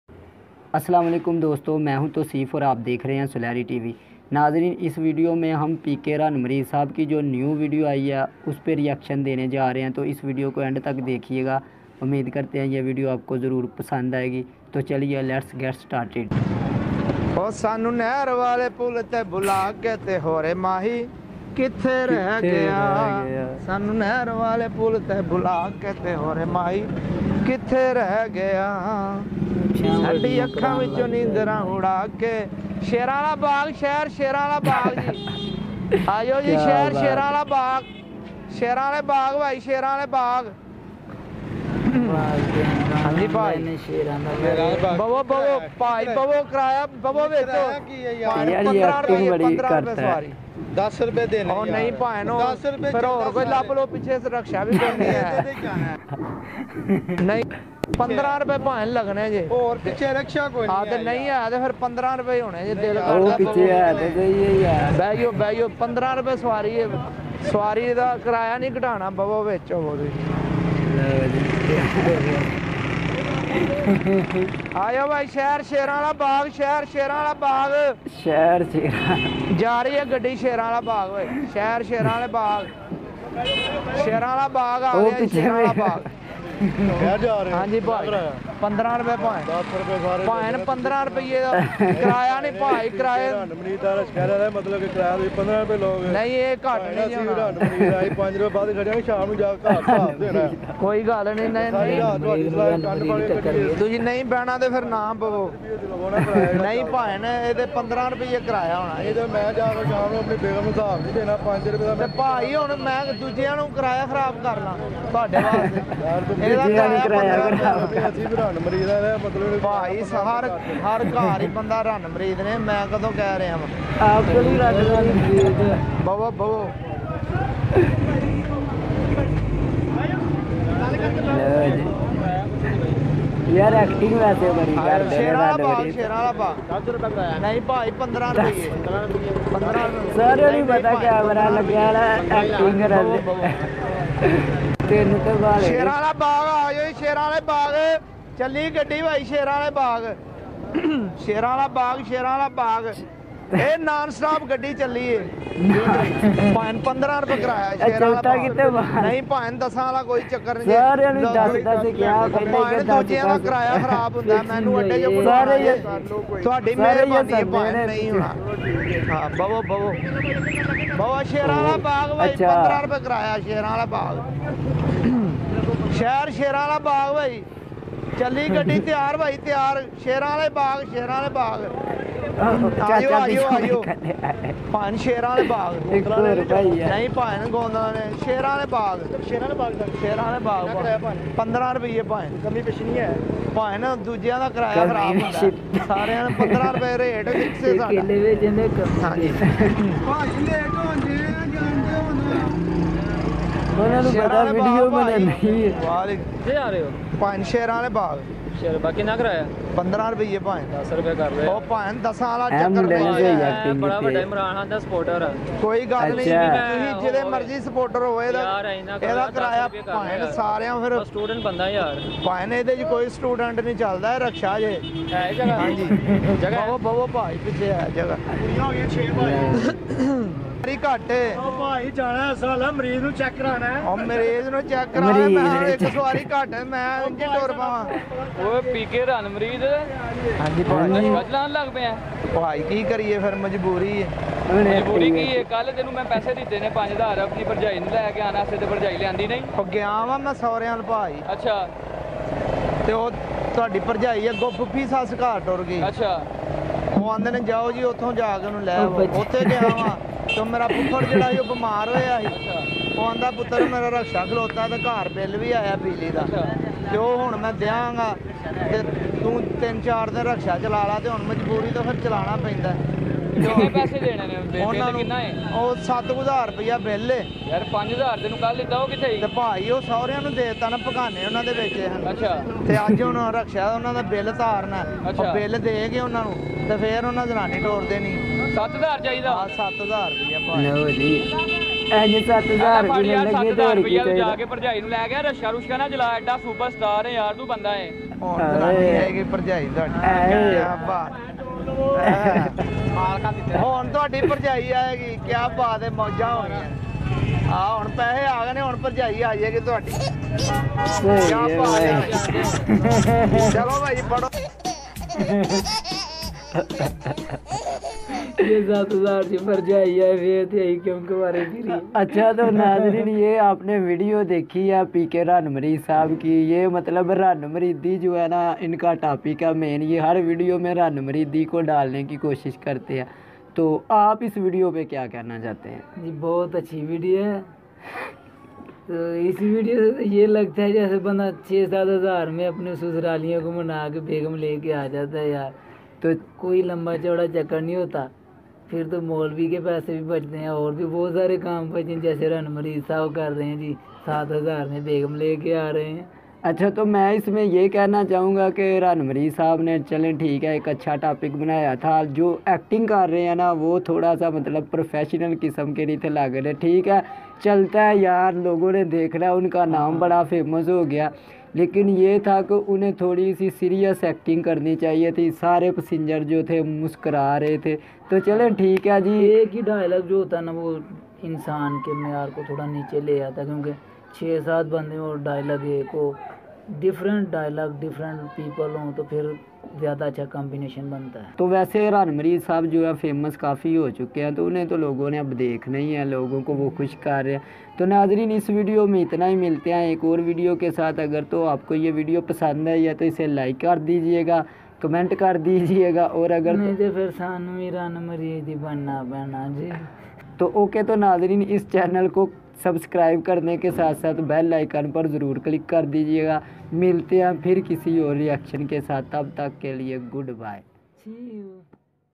PK Run Mureed new video 2018 | A jao wai Shairwala Bagh Karaya 15 | Solahri TV reaction
PK Run Mureed new video 2018 | A jao wai Shairwala Bagh Karya 15 Rupees:

This is a reaction video of famous Pakistani social media star PK run Mureed. I hope you will enjoy our honest reaction.